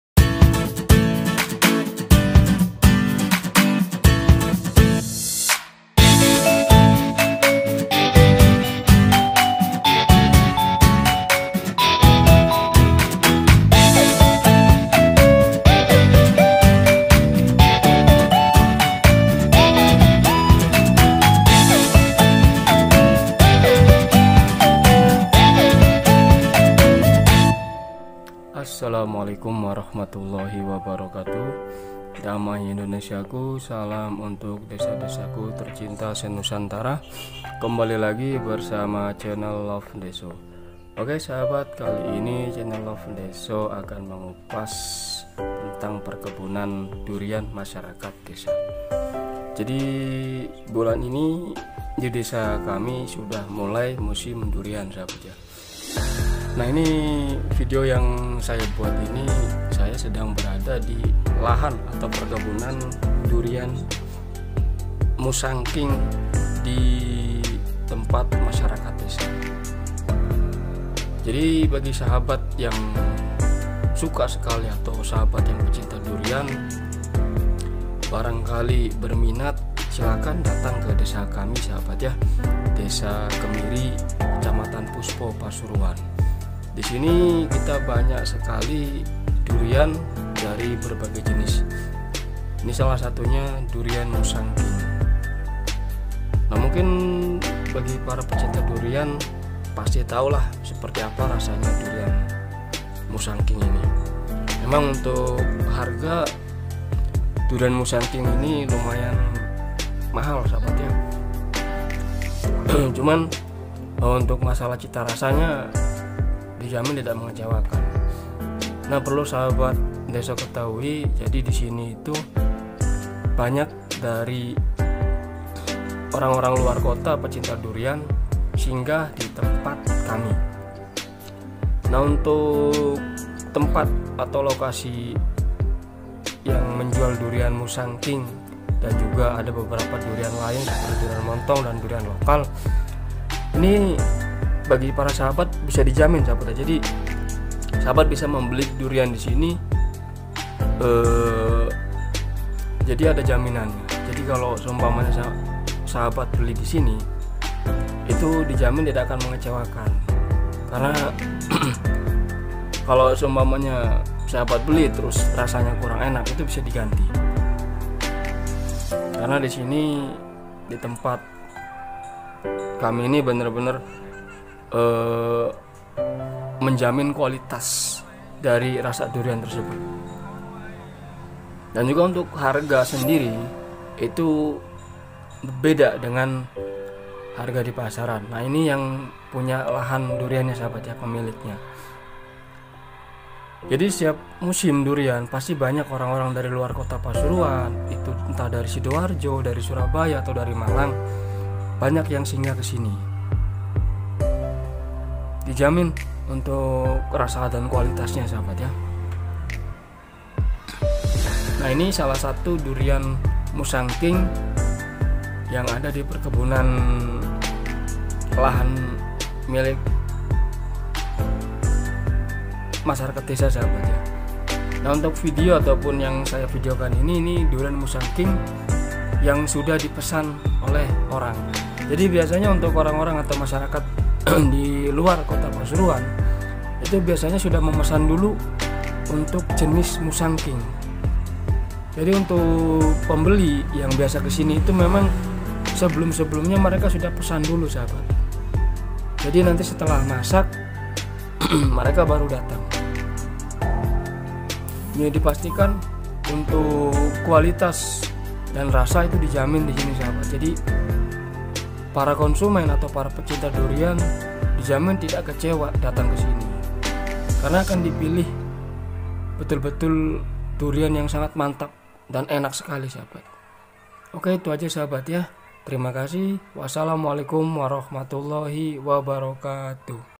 Perkebunan Durian Musang king
perkebunan Durian

love ndeso selalu mengupas apa apa saja yang ada di desa,kali ini kita mengulas tentang perkebunan Durian Musang king milik masyarakat desa,bagi pecinta durian pasti tau durian musang king,dan bagaimana lezat rasa nya, silahkan bagi para pecinta durian,singgah di desa kami,kami menjamin kualitas rasa dari durian nya.